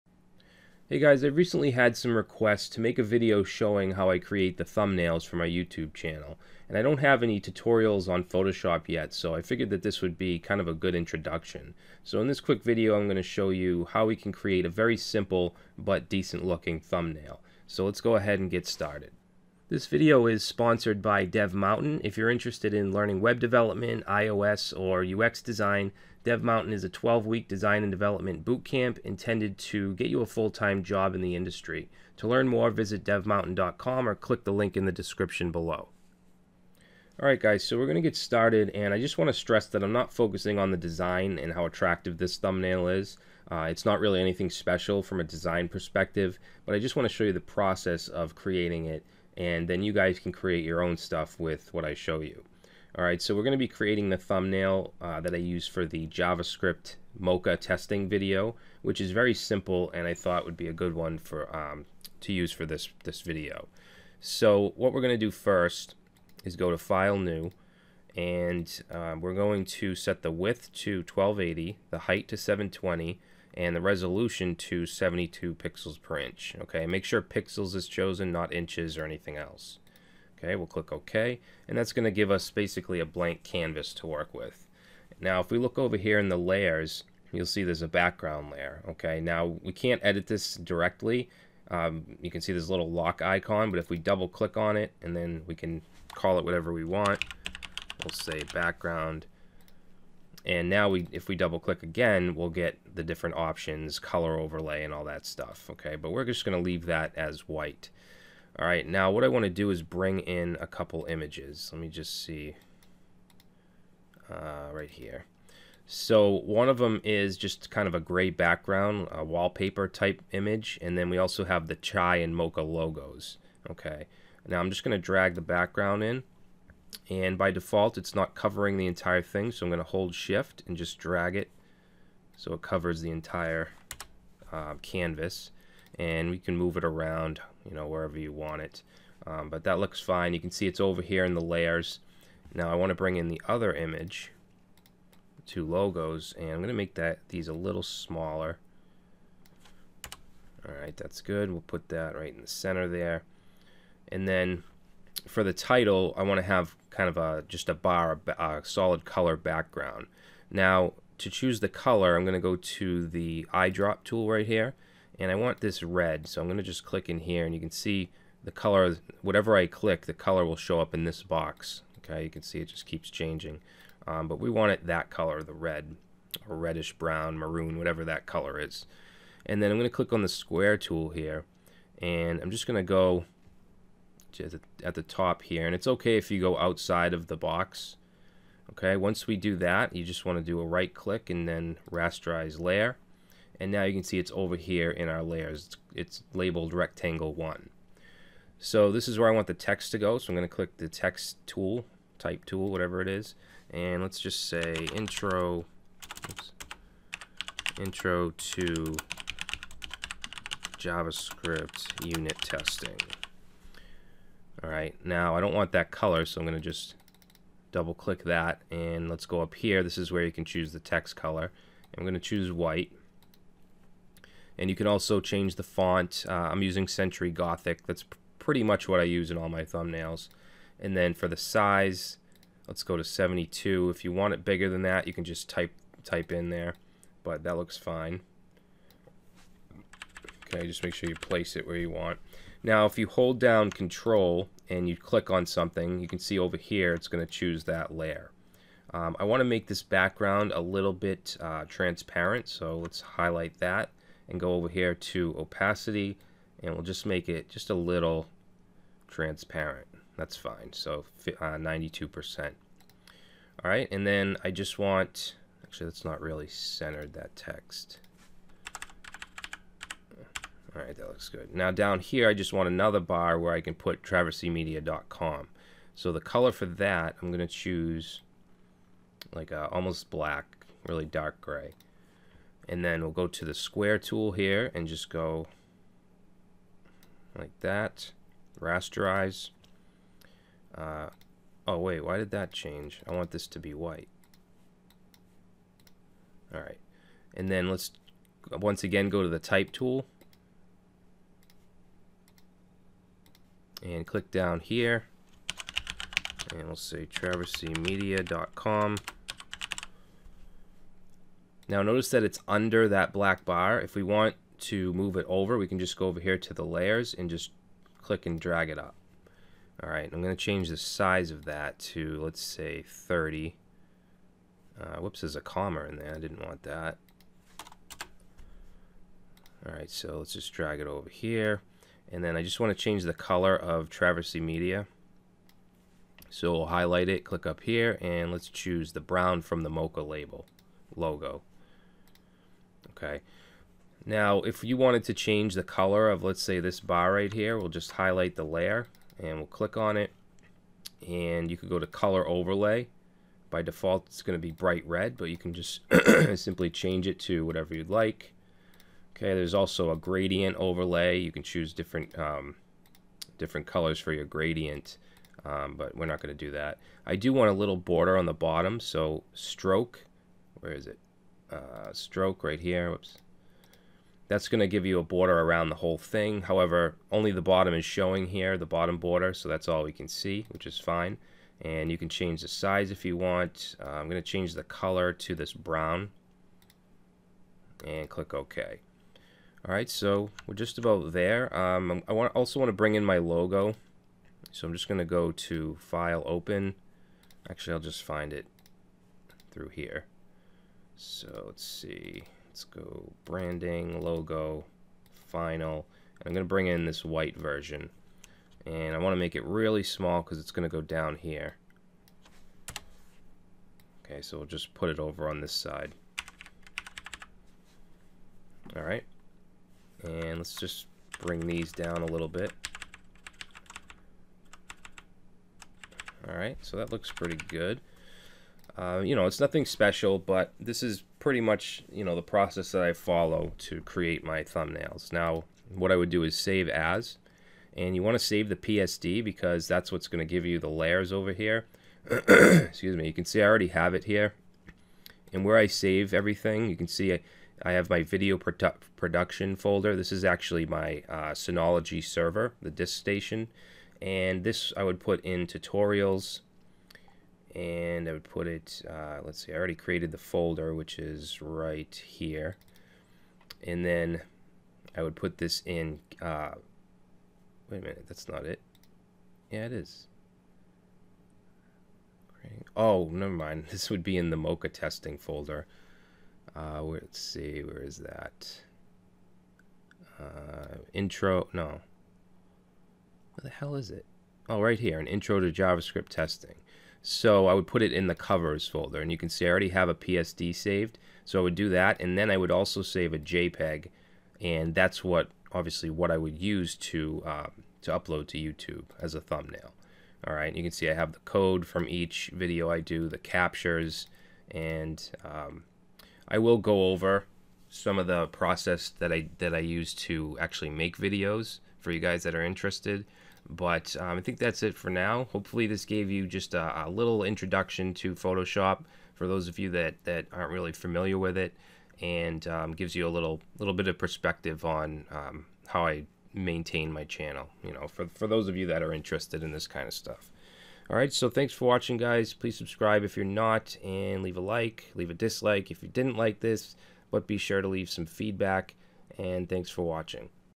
Creating Simple YouTube Thumbnails With Photoshop
In this video I will go through the process I use to create my YouTube channel thumbnails in Photoshop. This tutorial is very beginner friendly

SUPPORT: We spend massive amounts of time creating these free videos, please donate to show your support: